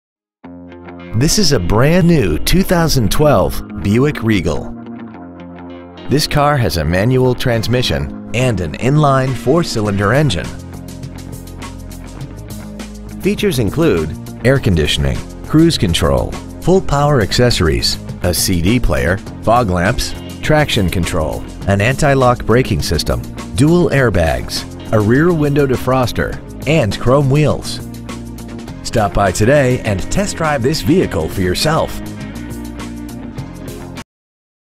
2012 Buick Regal Downers Grove IL 60515
Year : 2012
 Make : Buick
 Model : Regal
 Engine : 2.0L 4 Cyl. Turbocharged
 Trans . : 6 Speed Manual
 Exterior : Carbon Black Metallic
 Miles : 2

 Stock : 125224